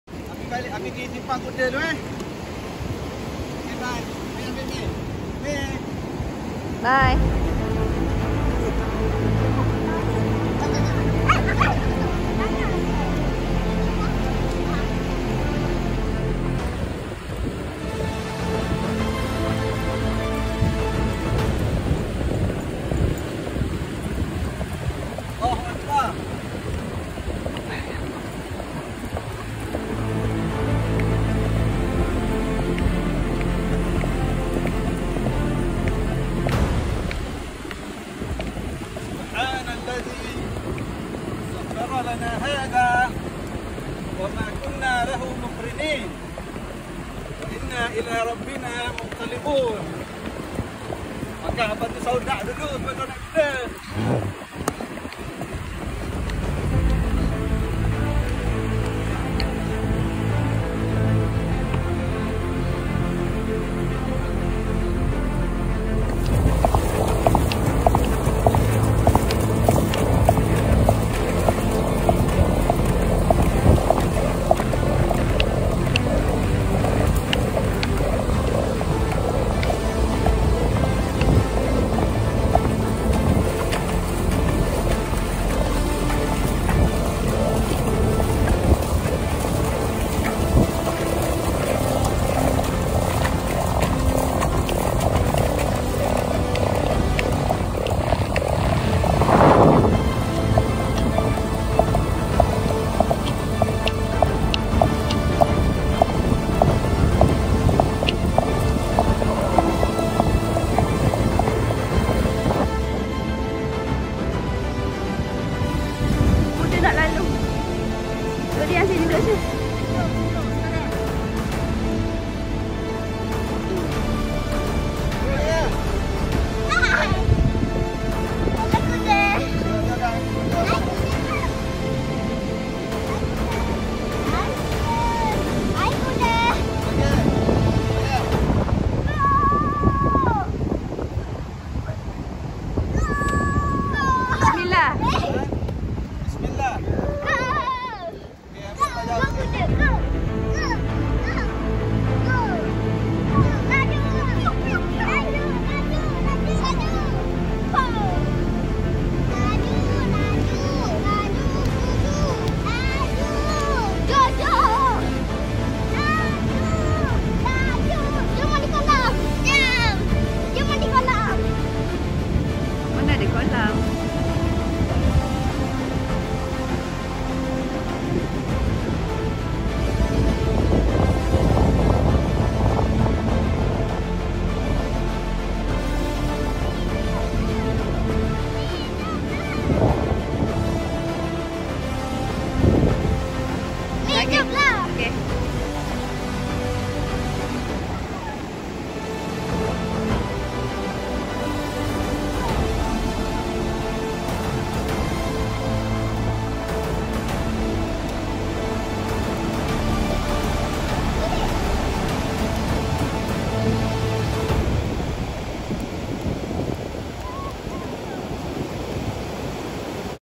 HORSE RIDING AT CHERATING BEACH….AWESOME !! 🤩🤩🤩
Sambil ride di tepian pantai….menikmati belaian angin & bunyi ombak 🥰🥰🥰